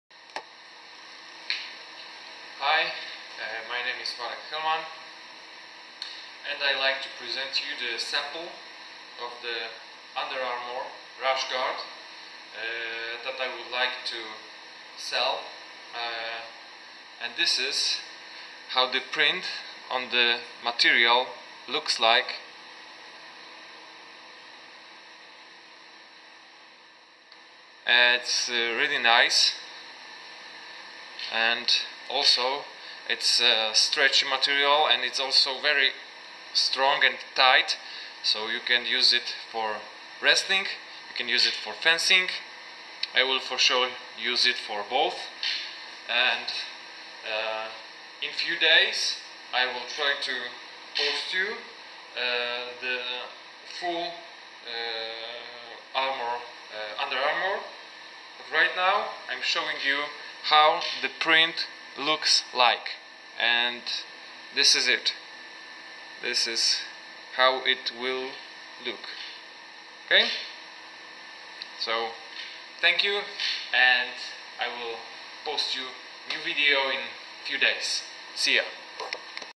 Rashguard - 1590 Doublet - sample
Short presentation of print sample. Soon full UA will be presented.